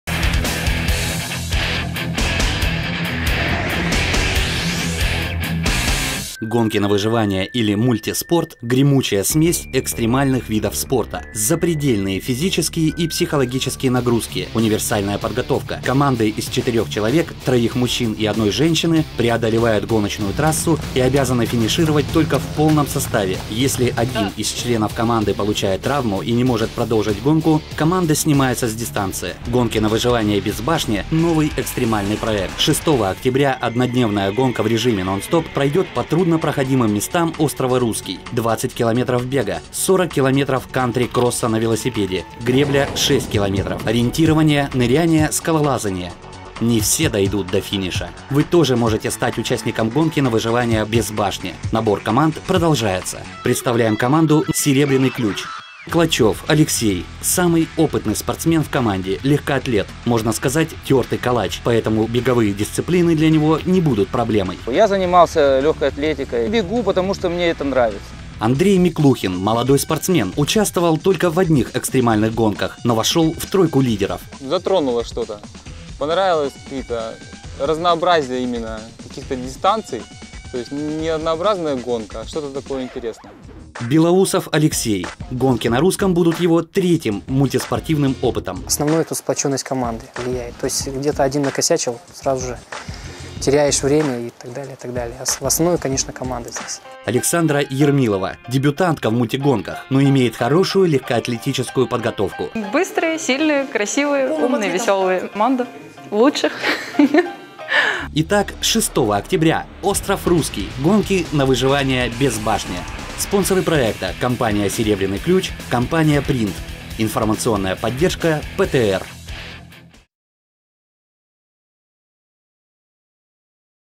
гонки на выживание "Без башни"
гонки на выживание на острове Русский во Владивостоке